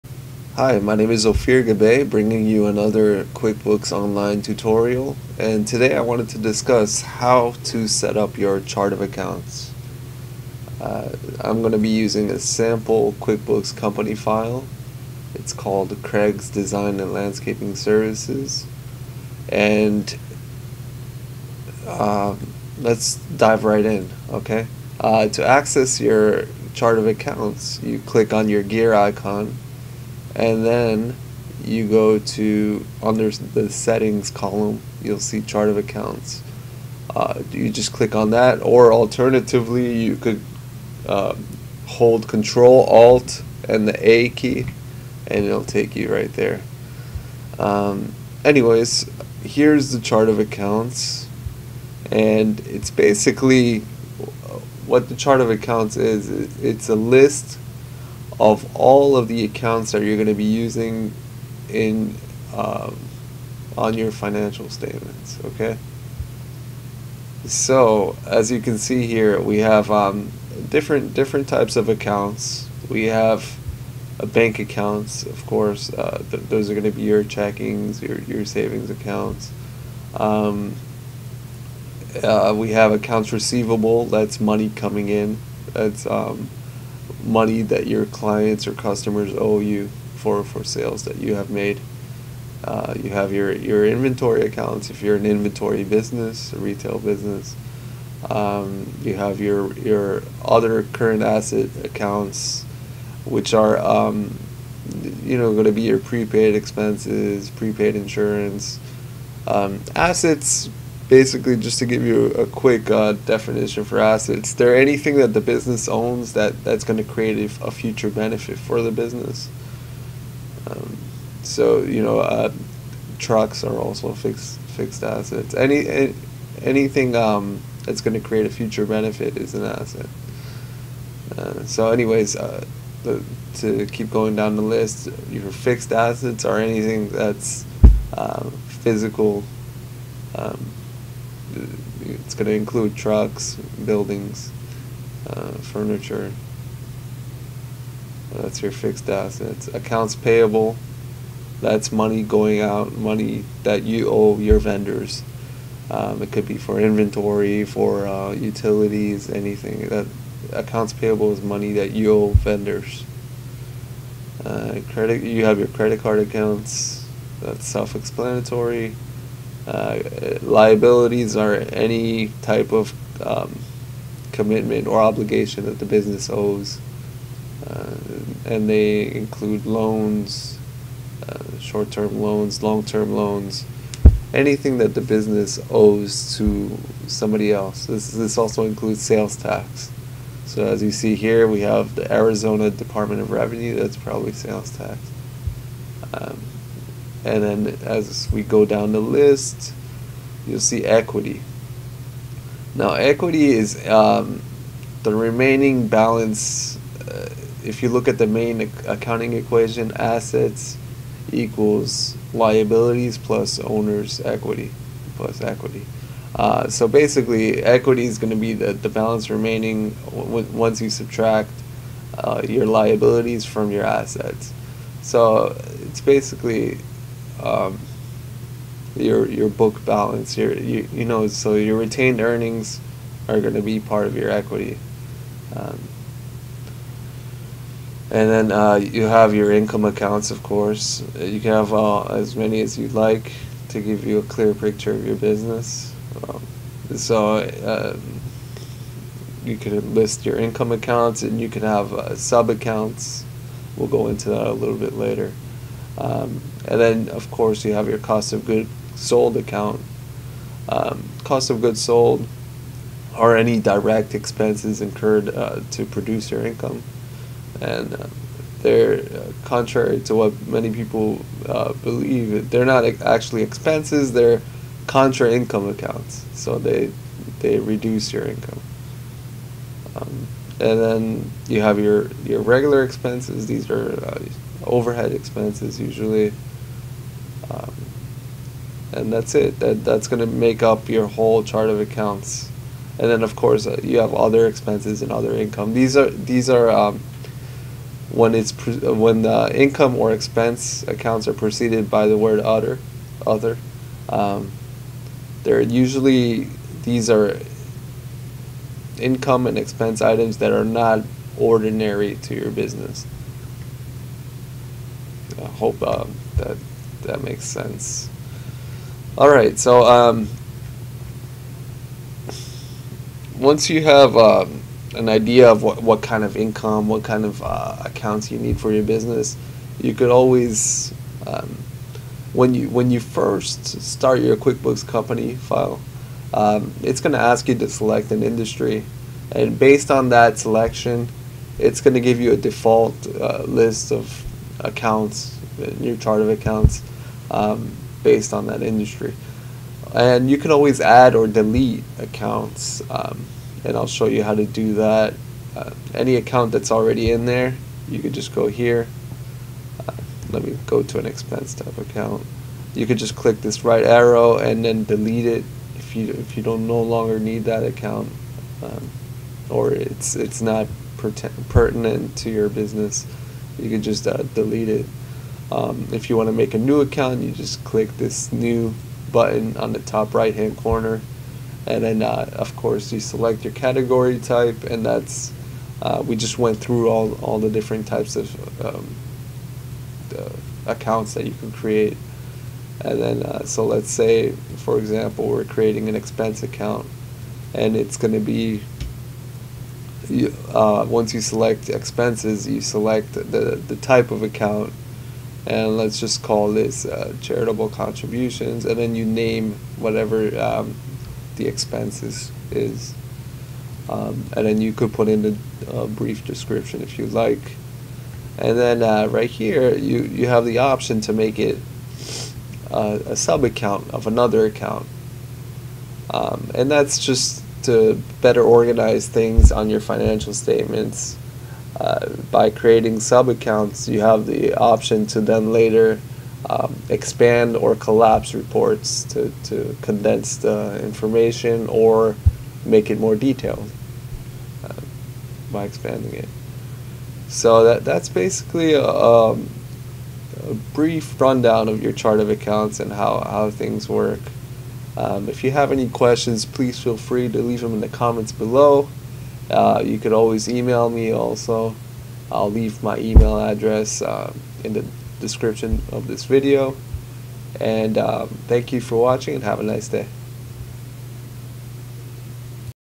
What is the Chart of Accounts? - QuickBooks Online Tutorial
In this video, I give a brief overview of what your chart of accounts is and how it is used in QuickBooks Online. I then go into the different account types with examples of each.

for more information on the services we offer and how we can help you.